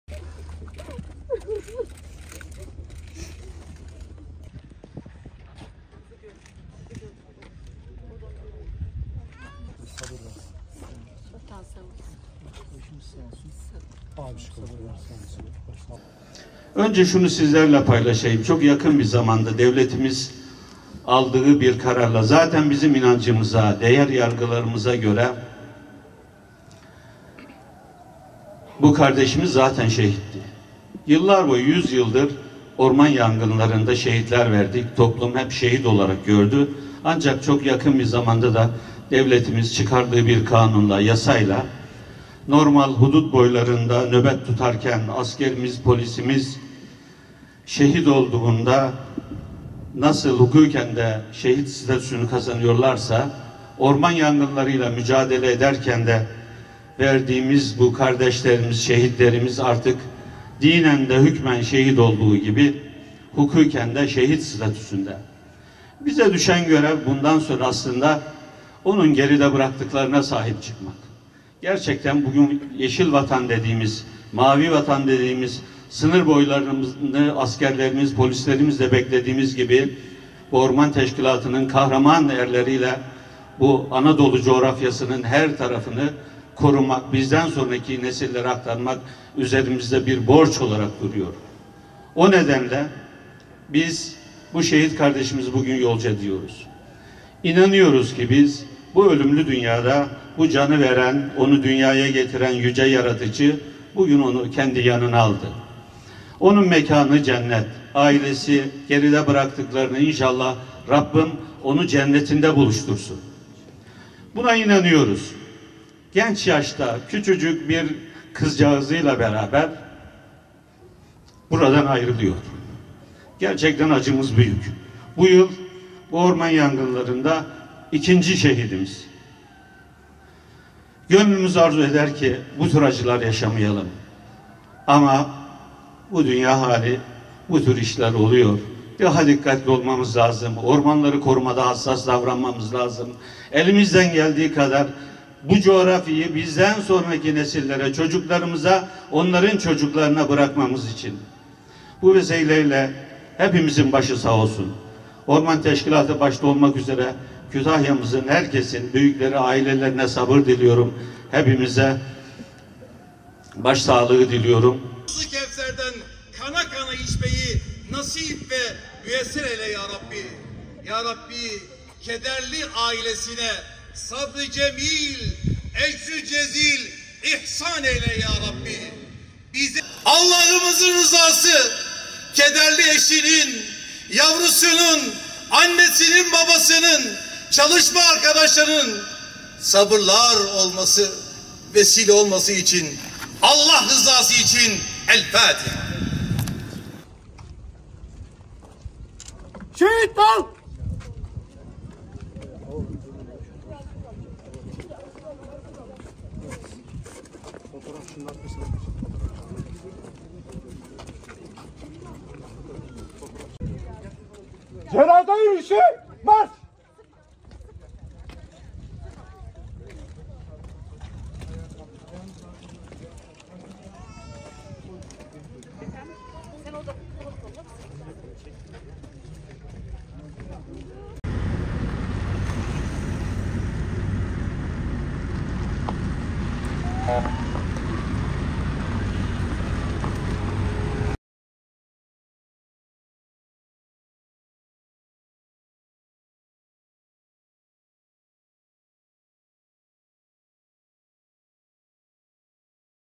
Mesai arkadaşları şehit Yıldırım’a veda etti
Kütahya’da çıkan orman yangınına müdahale ederken yaralanan ve tedavi gördüğü hastanede şehit olan Orman İşletme Müdürlüğü Şefi Murat Yıldırım (36) için Orman Bölge Müdürlüğü’nde tören düzenlendi